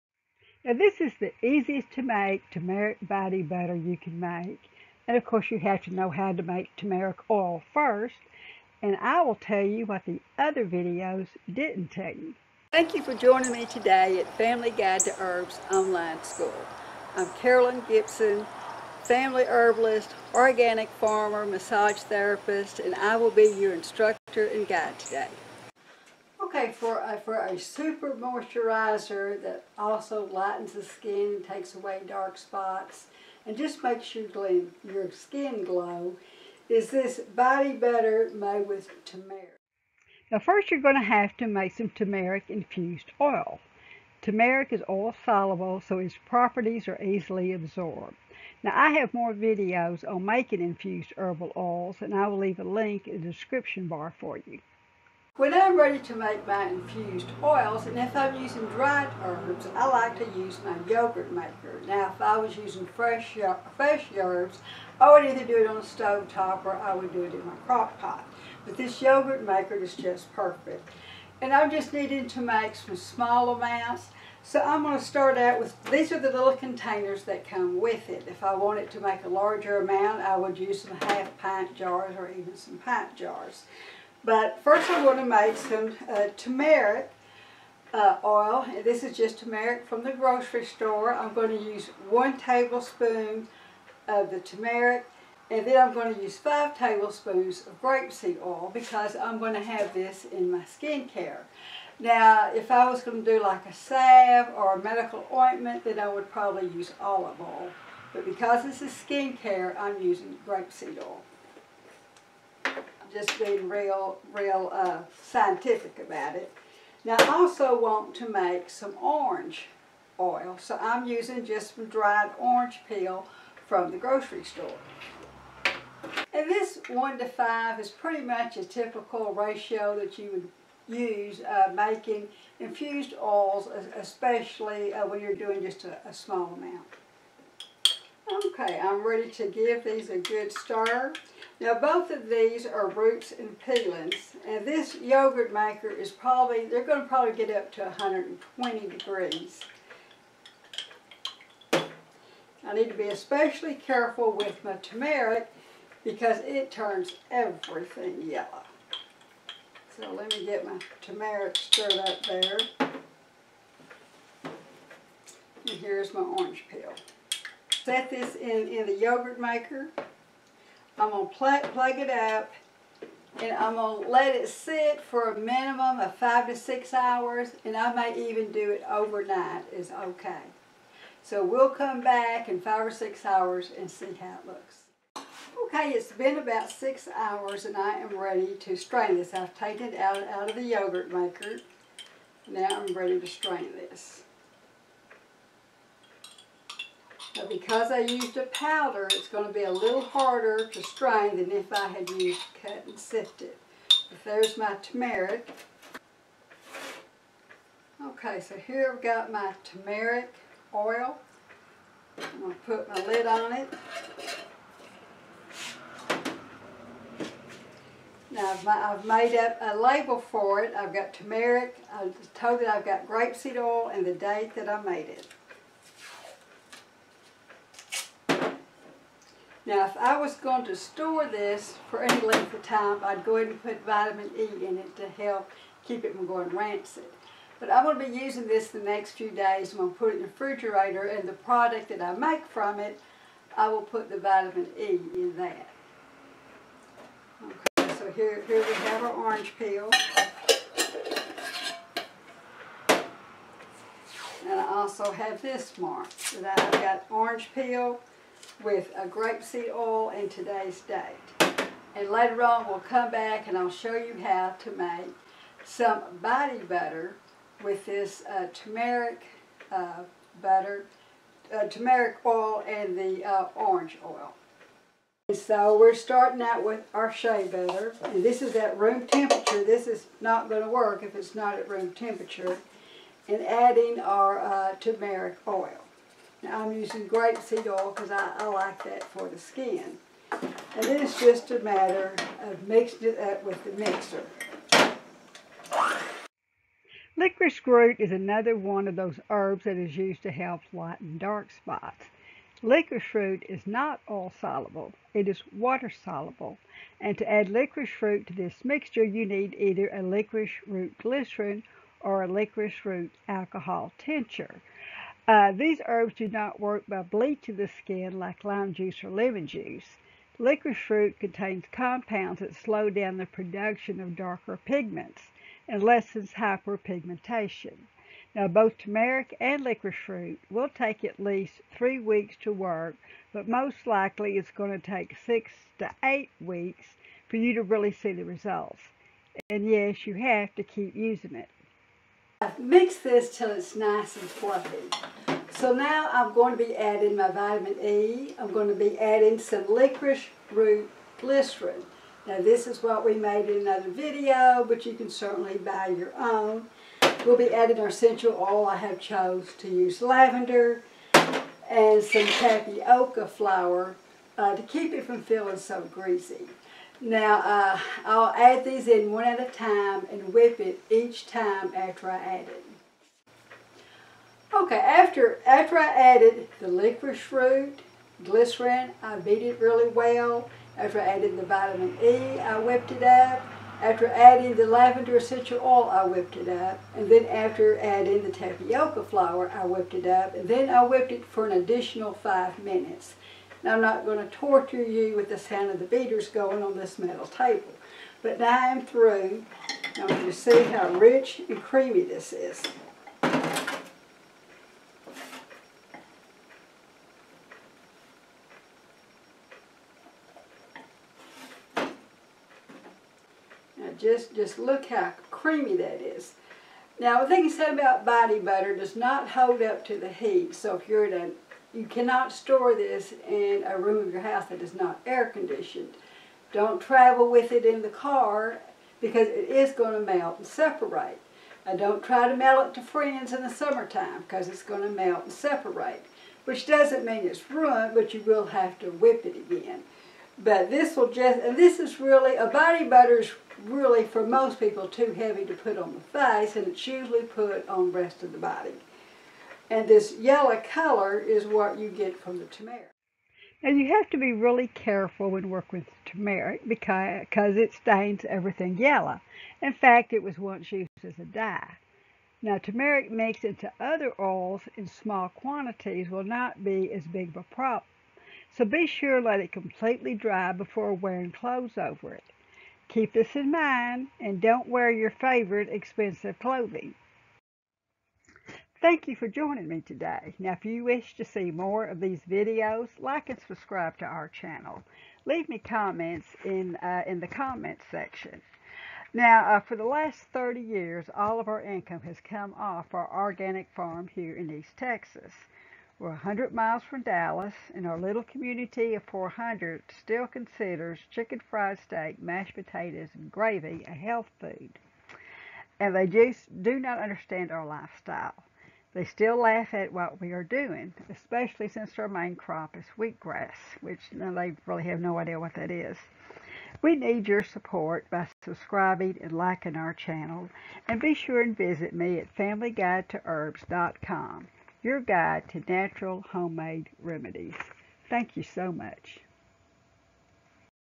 The Easiest to Make Turmeric Body Butter with Licorice Root and How to Make Turmeric Oil
How to Make Turmeric Body Butter with Licorice Root.  How to Make Turmeric Oil. There are many videos out there on how to make Turmeric Body Butter to reduce dark spots and to even the skin.  What they did not tell you was it is going to take a minimum of 3 weeks to see any results and most likely 6-8 weeks. And yes, you will have to continue to do it.  Did you know that Turmeric was once used as a yellow dye?  You will need to be very careful working with Turmeric to avoid staining everything yellow.  Once it is incorporated into a body butter it will not be as much as a problem.  I would let it completely dry before dressing.
Licorice Root glycerin or alcohol tincture is added to this body butter recipe.
Both of these herbs contain compounds that work to reduce hyperpigmentation.  They do not bleach the skin like lime or lemon juice.

Infused Turmeric Oil
1 tablespoon dried Turmeric Powder
5 tablespoons of Grapeseed Oil
Heat in a yogurt maker, or heat over the stove top keeping the temperature between 100-120°f.

Turmeric Body Butter
4 oz. Shea Butter at room temperature
½ oz. Turmeric Infused Oil
1/8 teaspoon of Tapioca flour or Arrow Root
¼ teaspoon essential oil
1/4 teaspoon Vit E
(250 IU of Vitamin E)
Optional: 1 tablespoon of Licorice Glycerin Tincture